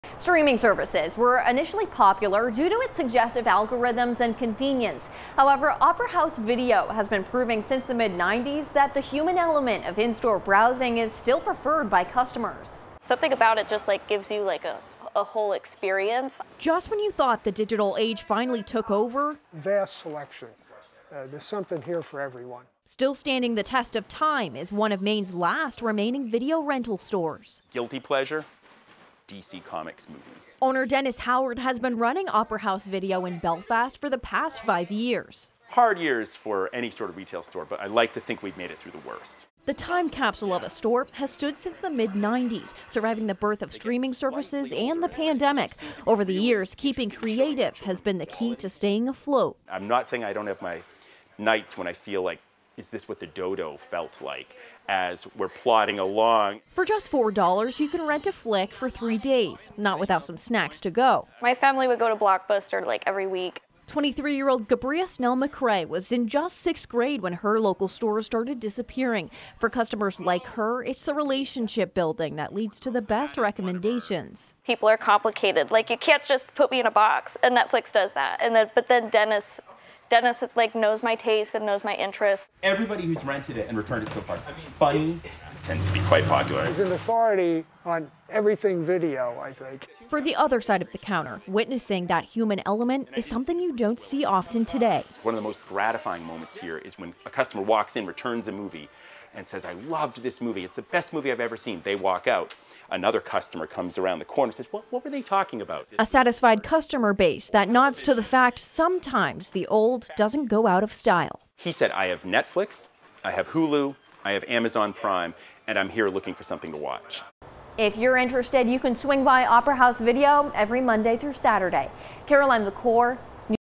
Open since the mid-90s, this Maine video rental store is standing the test of time
Opera House Video is now known as one of Maine's last remaining video rental stores, surviving both the birth of streaming and the COVID-19 pandemic.